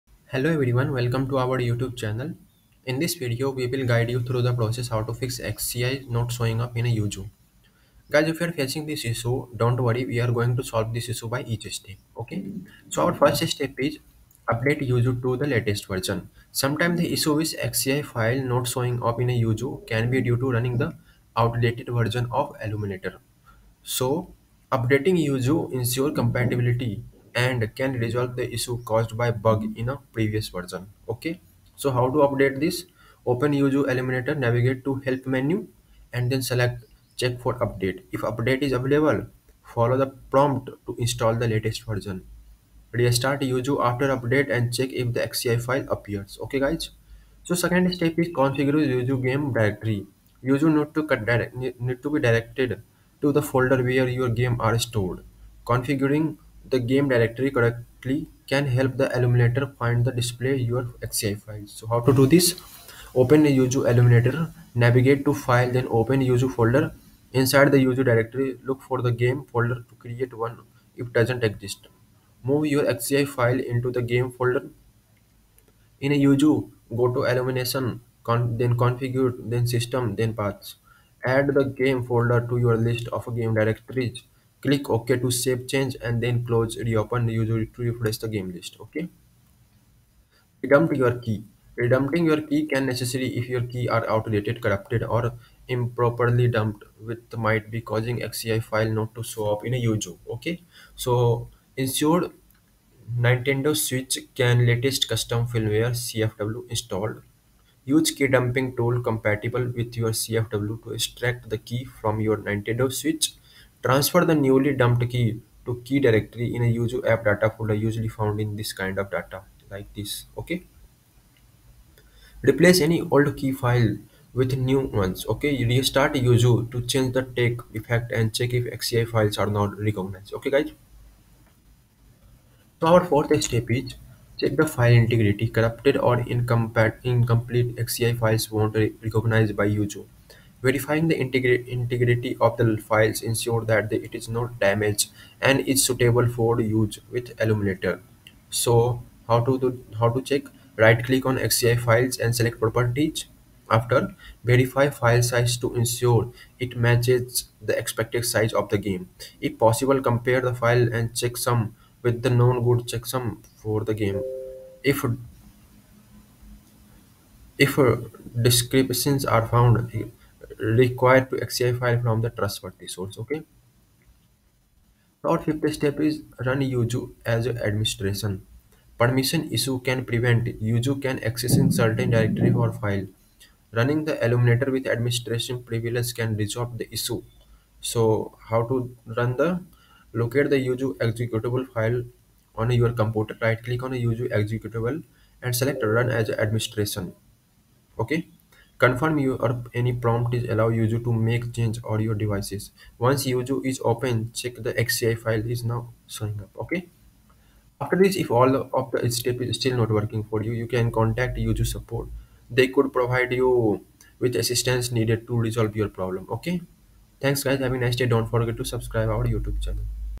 How To Fix XCI Not Showing Up In Yuzu
In this video we will guide you through the process How To Fix XCI Not Showing Up In Yuzu
To fix XCI not showing up in Yuzu, you can update Yuzu to the latest version, configure Yuzu’s game directory, and redump your keys. Additionally, you can check file integrity, and run Yuzu as administrator

00:00  Introduction
00:13  Solution1:Update Yuzu To The Latest Version
00:47  Solution2:Configure Yuzu’s Game Directory
01:30  Solution3:Redump Your Keys
02:10  Solution4:Check File Integrity
02:54  Solution5:Run Yuzu As Administrator
03:27  Solution6:Contact Yuzu Support